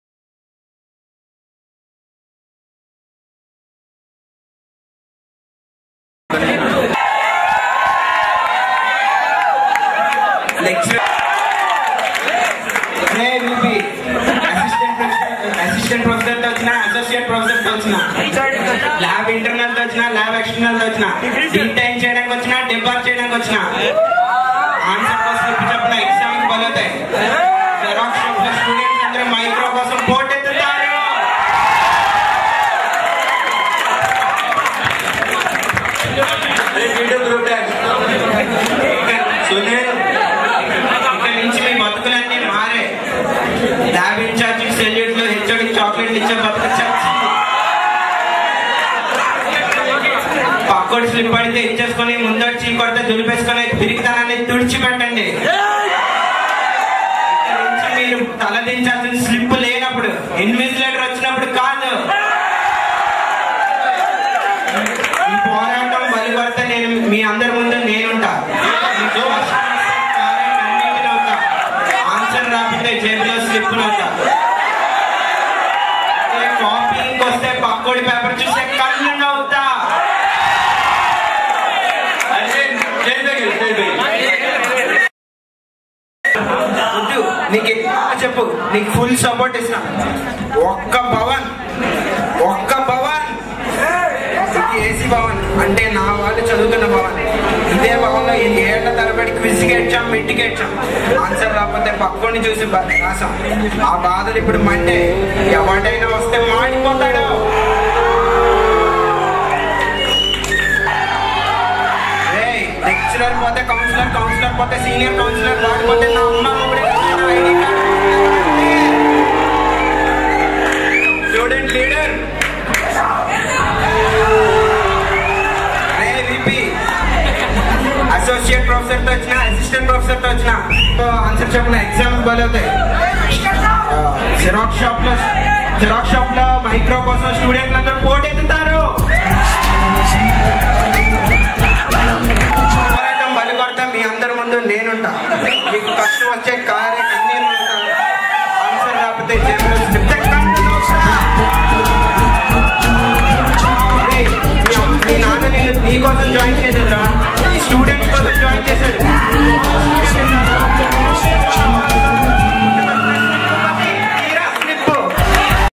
Slippupathi-Chatrapathi spoof by GITAM Fresher-Stage performance
This is an unplanned performance in Freshers by a Prabhas Fan from GITAM,Vizag.
The video is slightly edited for user experience...